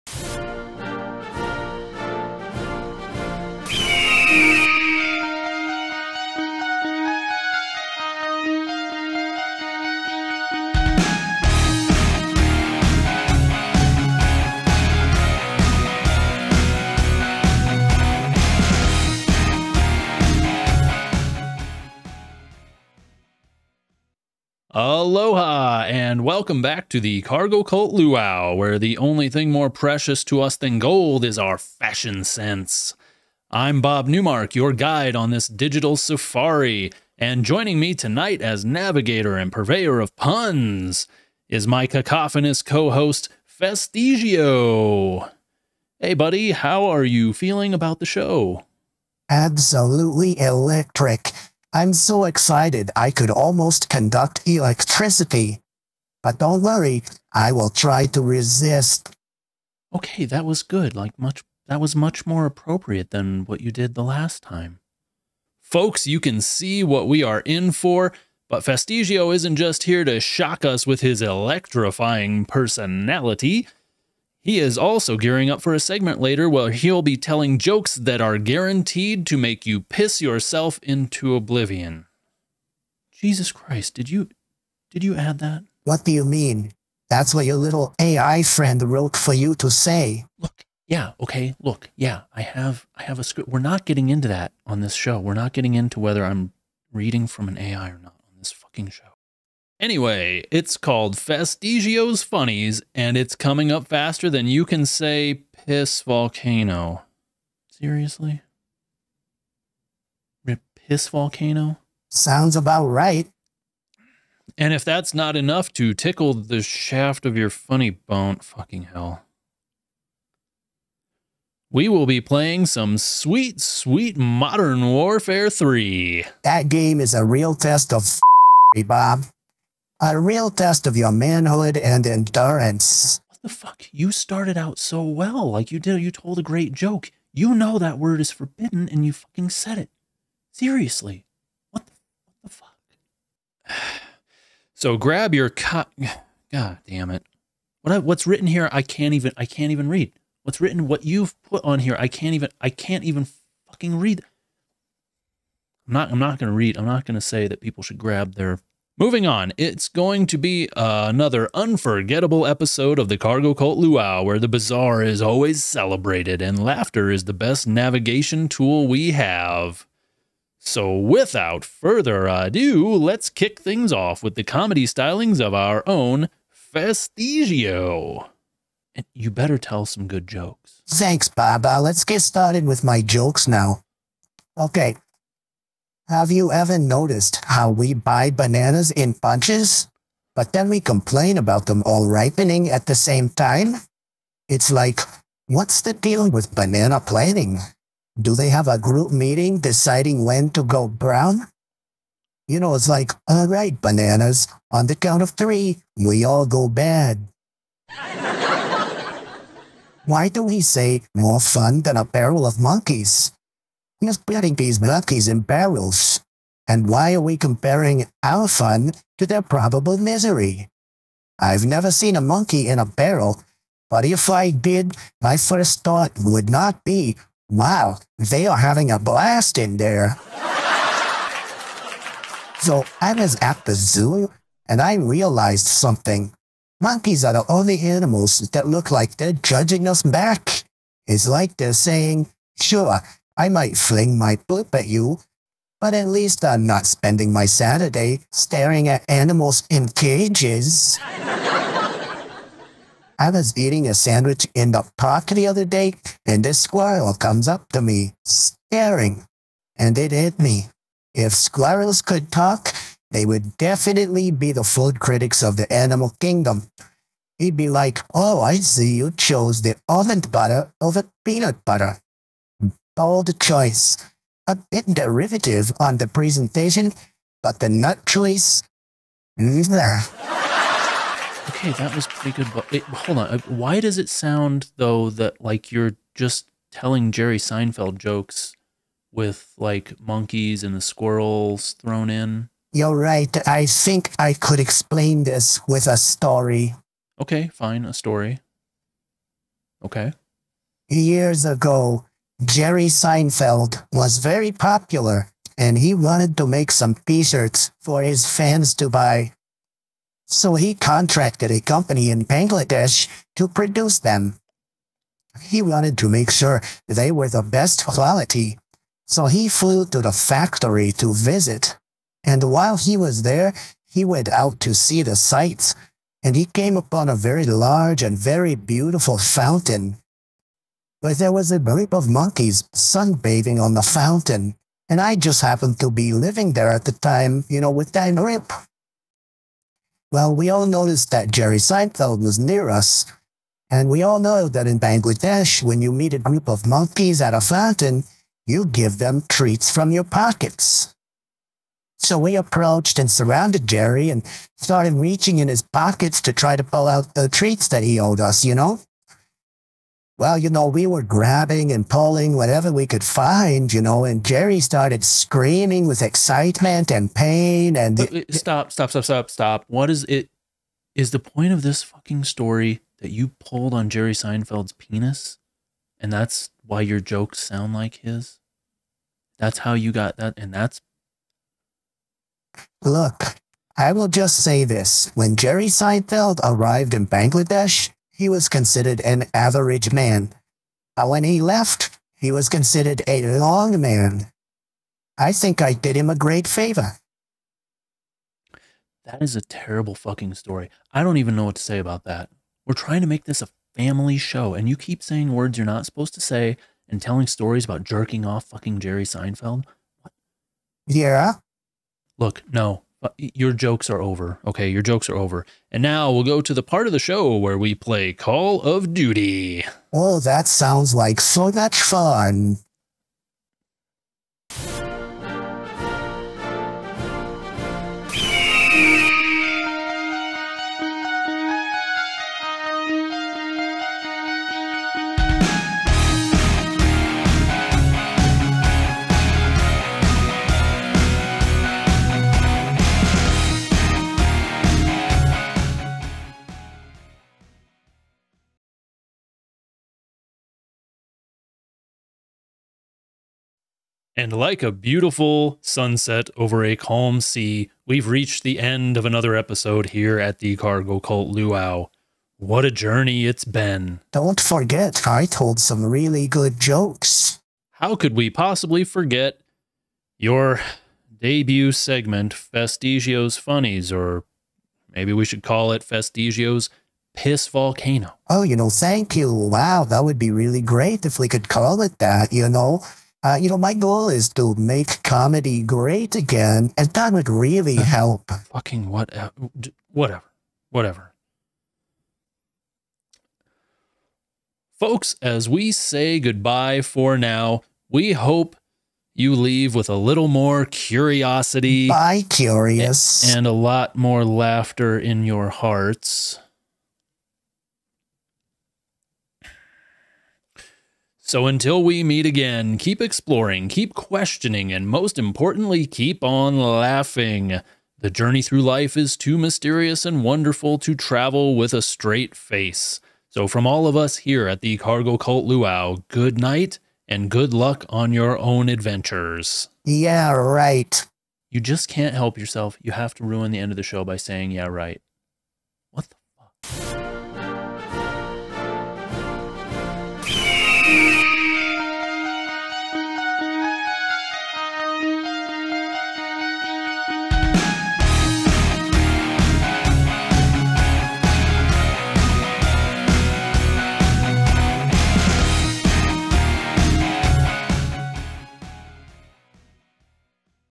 Festigio's Funnies / The Cargo Cult Luau Episode 1
Festigio the bastard monkey tells some jokes.
The Cargo Cult Luau Episode 1.
This is the Episode with the gaming content removed, so it can be enjoyed in more compact form. To see it live, hit the notification button for when the livestreams are...live. Or just wait for the edited upload.